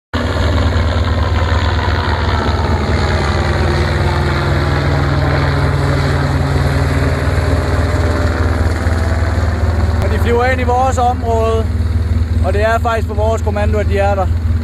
forestfires Dansk brandhold i Sverige. Samarbejde med helikoptere.
Samarbejde mellem det danske hold og brandslukningshelikopter med vandpose.